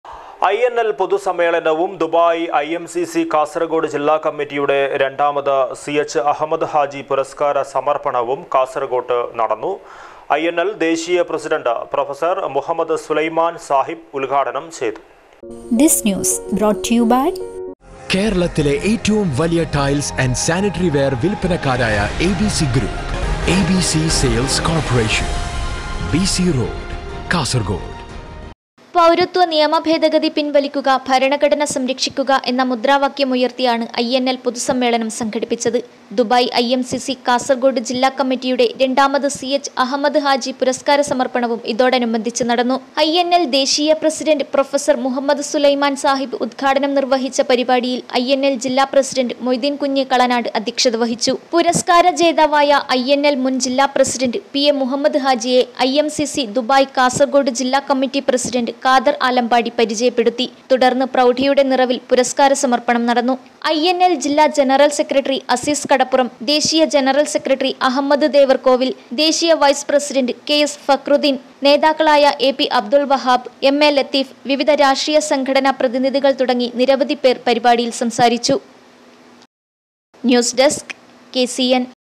സി.എച്ച്. അഹമ്മദ് ഹാജി പുരസ്‌ക്കാര സമര്‍പ്പണവും കാസര്‍കോട്ട് നടന്നു.
ഐ.എന്‍.എല്‍. പൊതു സമ്മേളനവും ദുബായ് ഐ.എം.സി.സി. കാസര്‍കോട് ജില്ലാ കമ്മിറ്റിയുടെ രണ്ടാമത് സി.എച്ച്. അഹമ്മദ് ഹാജി പുരസ്‌ക്കാര സമര്‍പ്പണവും കാസര്‍കോട്ട് നടന്നു. ഐ.എന്‍.എല്‍. ദേശീയ പ്രസിഡന്റ് പ്രൊഫസര്‍ മുഹമ്മദ് സുലൈമാന്‍ സാഹിബ് ഉദ്ഘാടനം ചെയ്തു.